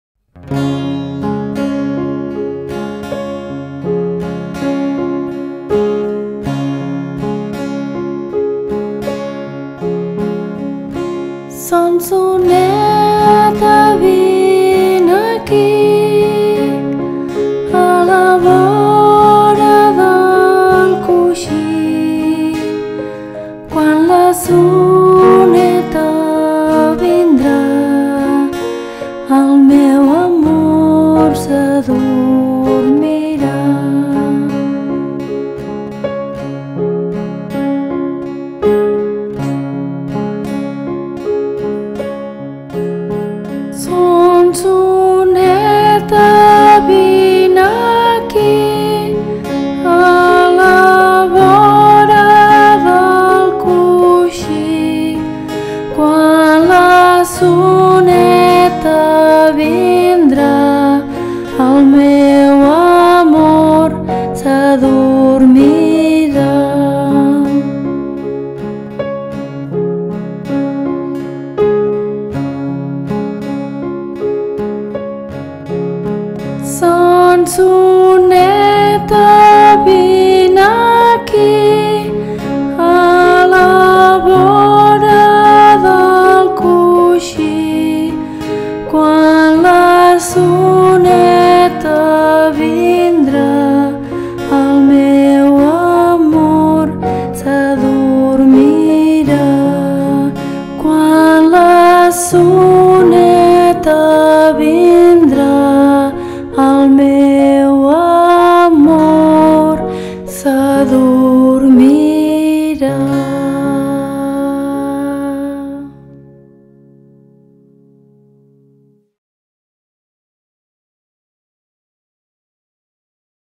Nanes per fer nones
Capítol pilot per a la producció de petites píndoles enfocades als més petits de la casa. Plantejats per emetre's prèviament als noticiaris nocturns.

Capítulo piloto para la producción de pequeñas animaciones  enfocadas para los más pequeños de la casa, planteados por emitirse previos a los noticiarios nocturnos.